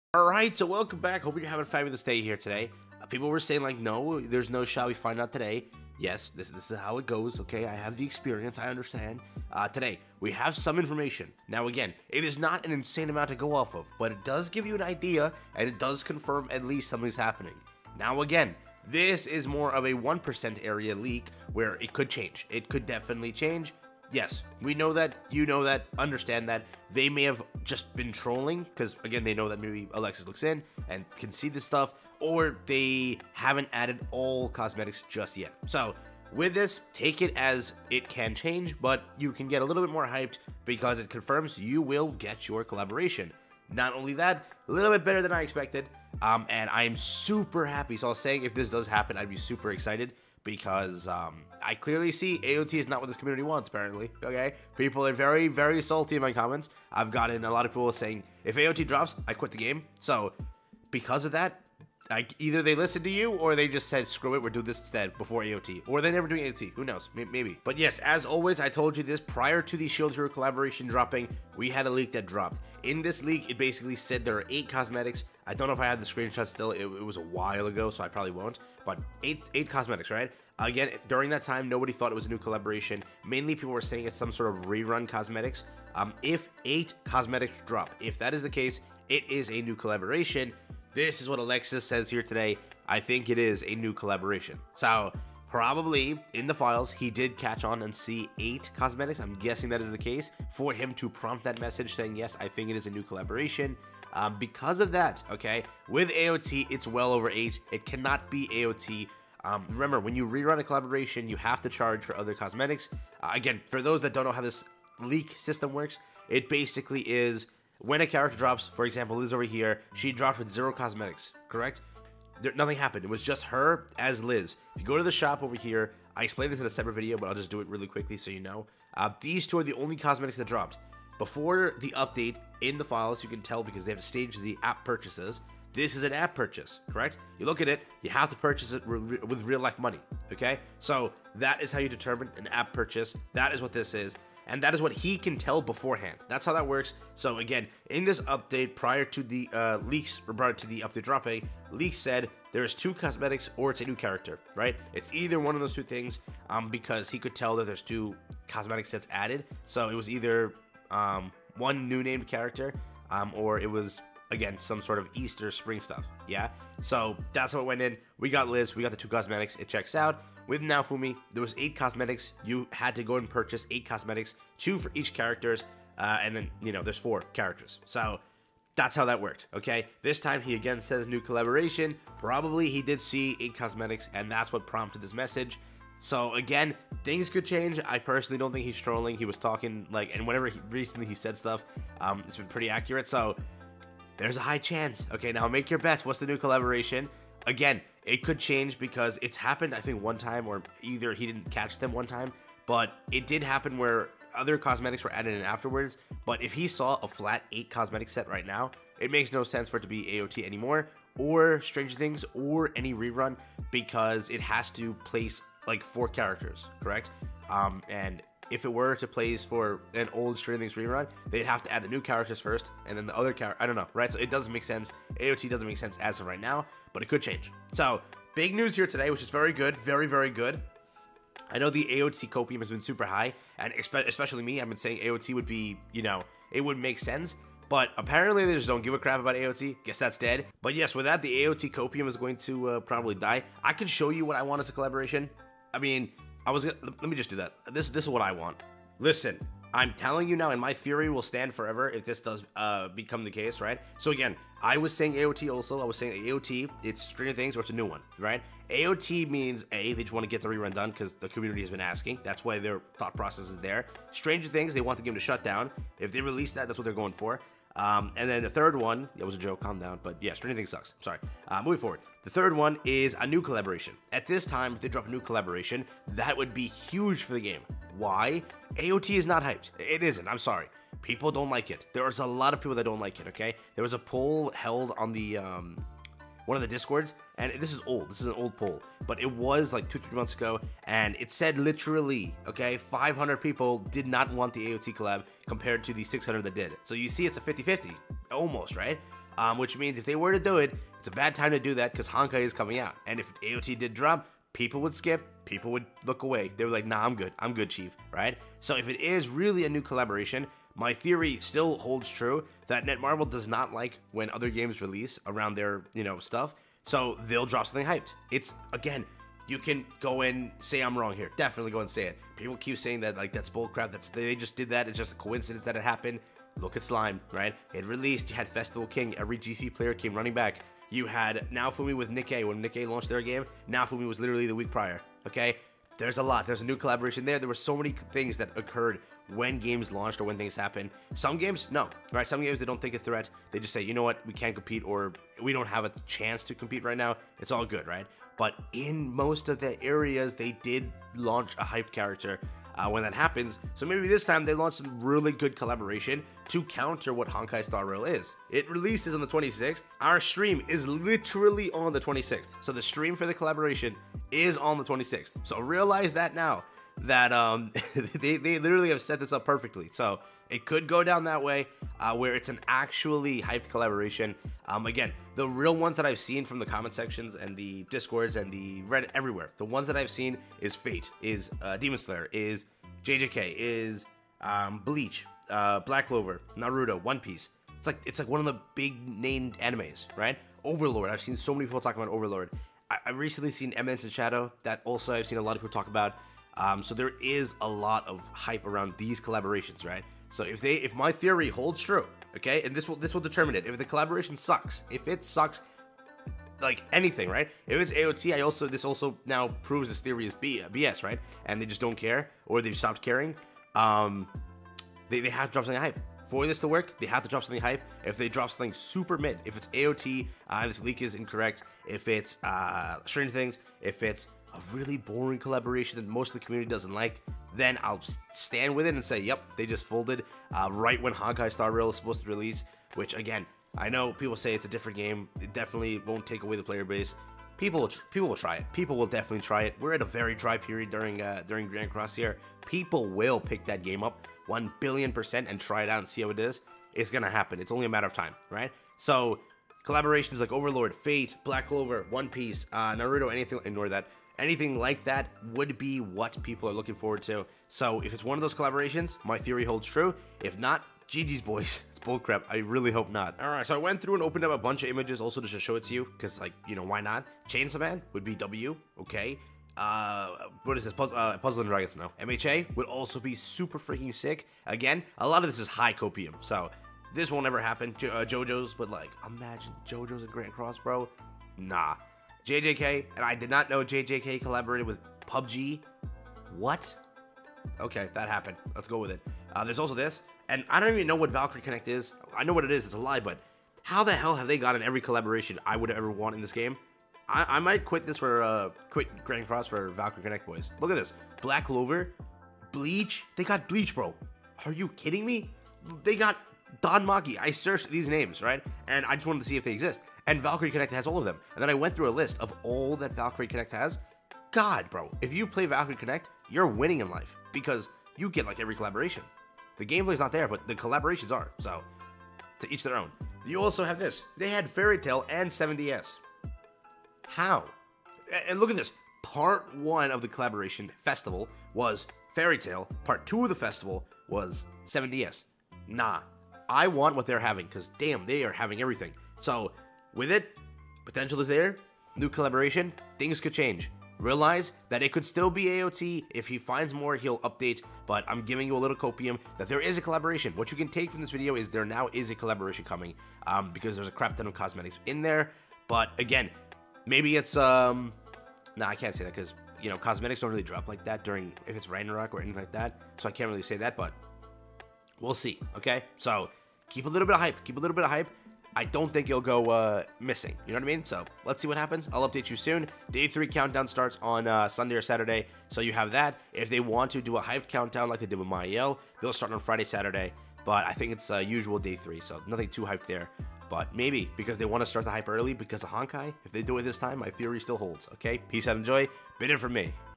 *SO BOIS, WE GOT AN UPDATE ON THE COLLAB!!* & IT MIGHT BE BETTER THAN WE EXPECTED (7DS Grand Cross)
Mael is finally here after such a long wait! He is here as part of the Global 3 Year Anniversary and is bringing the Goddess meta back!

Freya arrived as part of the last Ragnarok update as well as a brand new demonic beast, Nidhogg!

Purgatory Meliodas, aka Trillion Dark Meliodas was the New Year Festival 2023 unit. For the 3.5 Anniversary Festival we got a Light Attribute Escanor, The One Ultimate. Its bittersweet, but it's the only way to give our goat the treatment he deserves.

Finally after more than 18 months of waiting we got a new collab for Grand Cross. It was The Rising of Shield Hero x Grand Cross. Naofumi, Raphtalia, Filo, & Kizuna made their appearance on that collab banner! Who knows which collab rerun will be next!

► Use My Epic Games Creator Code - GoGo